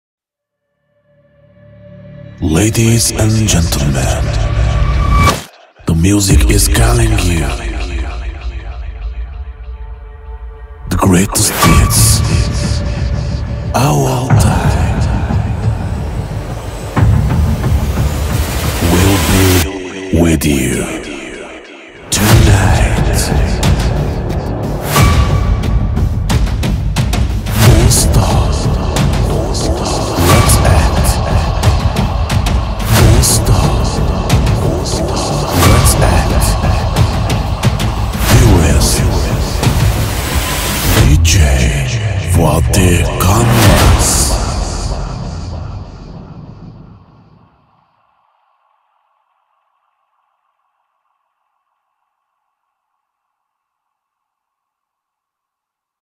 DJ FATİH KANMAZ - DJ Jingle
CLUB DJ'ler için hazırlanan DJ DROPS örneklerinden biri